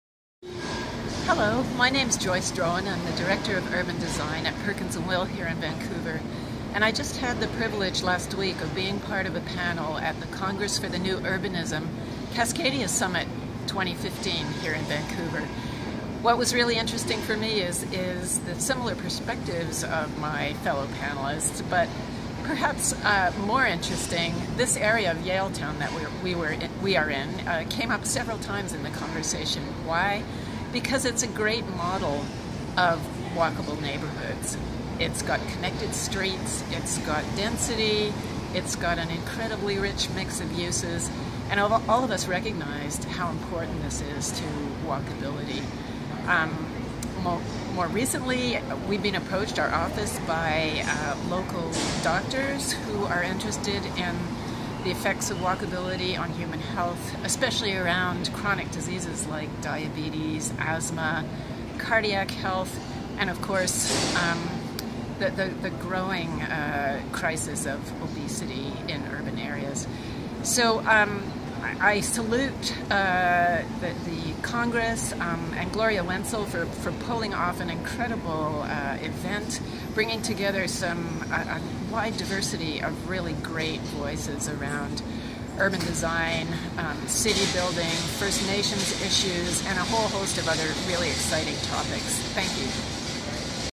CNU Cascadia's Vancouver Summit 2015 Joyce Drohan
Joyce Drohan Director of Urban Design -Perkins and Will-Vancouver Office